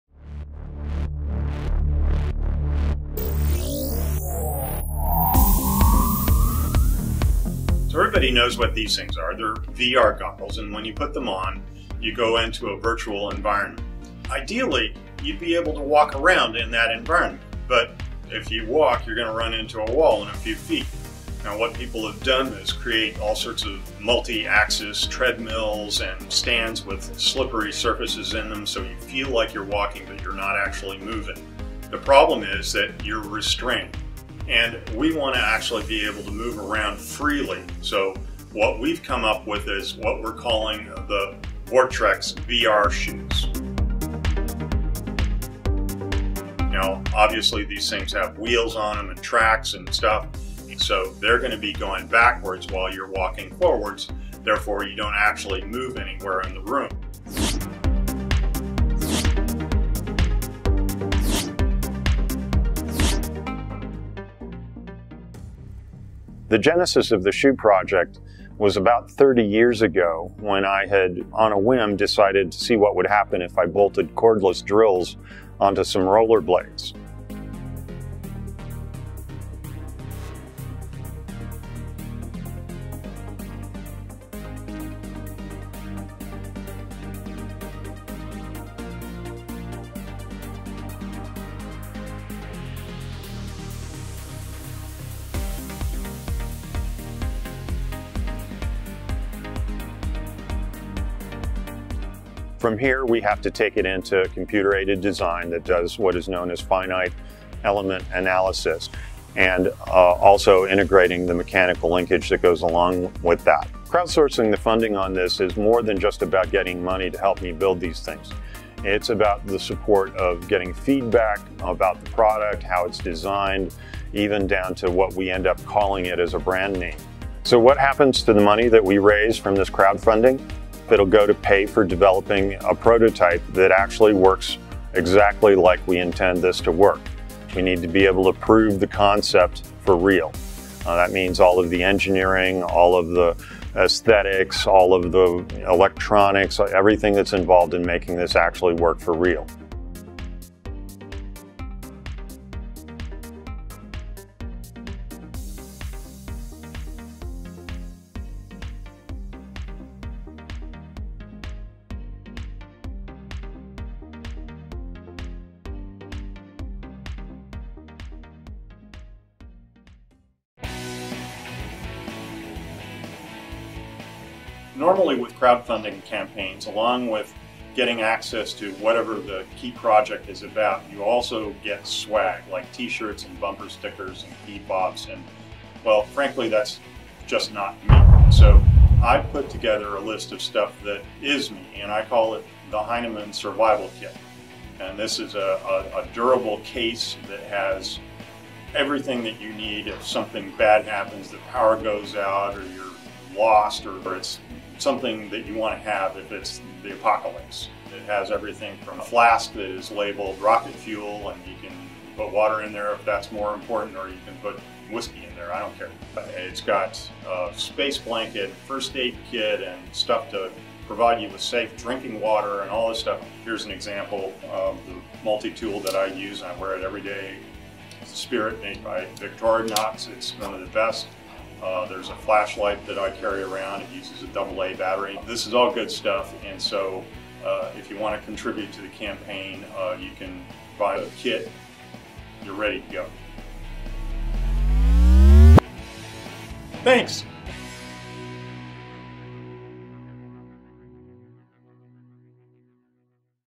Jamie Hyneman's Electric Shoes
Jamie Hyneman "walks" through the progress of an electric shoe project his team has been working on and the effect it could have on the VR world. (Even the real world!)